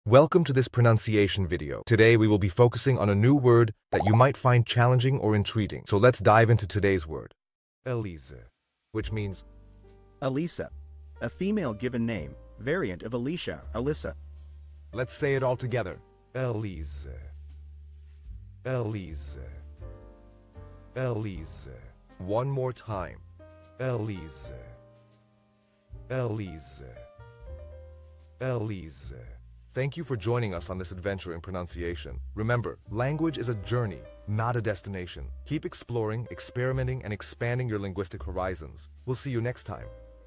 How to pronounce Alisa
Master the Pronunciation of 'Alisa Which Means Alisa' - which means : Alisa: Female Given Name, Variant of Alicia or Elissa. 📝 with @PortuguesePronunciation 🎓🗣️ - [Your Guide to Portuguese]

Learn to pronounce 'Alisa' perfectly in Portuguese with our easy and engaging tutorial by @PortuguesePronunciation! 📖📽️ In this comprehensive video, we provide you with native speaker examples 🗣️👂 and phonetic breakdowns 🎵 to ensure you get the pronunciation just right.

How to say Straight in Portuguese